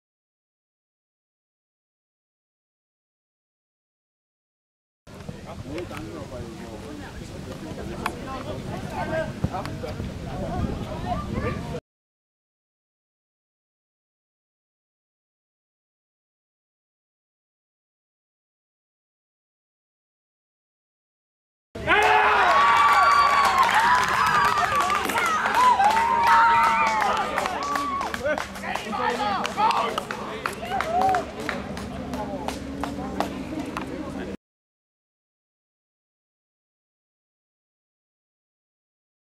Tor: SG Dynamo Dresden III 2:1 Dresdner SC 1898
Der 2:1 Führungstreffer für Dynamo Dresden III am 2. Spieltag in der Stadtoberliga Dresden. Nach einer Flanke kann Alexander Eißrich den Ball ins Tor schieben.